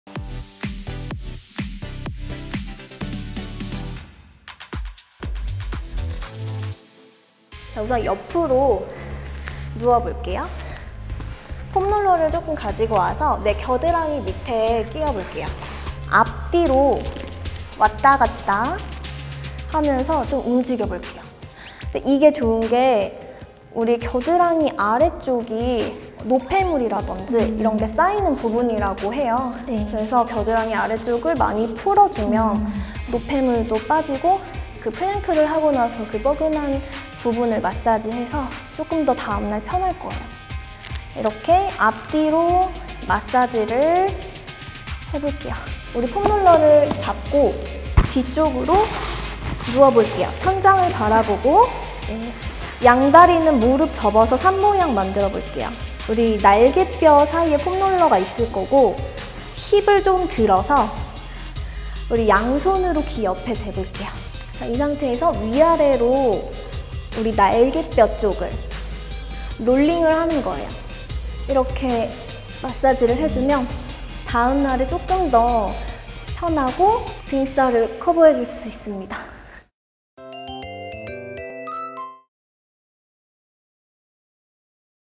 [머슬 퀸 홈 프로젝트] EP.4-6 WARM DOWN STRETCHING 폼롤러 스트레칭 [우정원/이하린]
홈트로 머슬 퀸이 될 수 있다? 머슬 퀸이 퀸 메이커로 변신!
어떻게 해야 할지 막막하기만 한 홈 트레이닝, 이제 걱정은 뚝!
일상적인 공간에서 평범한 방법으로 여러분을 머슬 퀸으로 변신시켜 드립니다!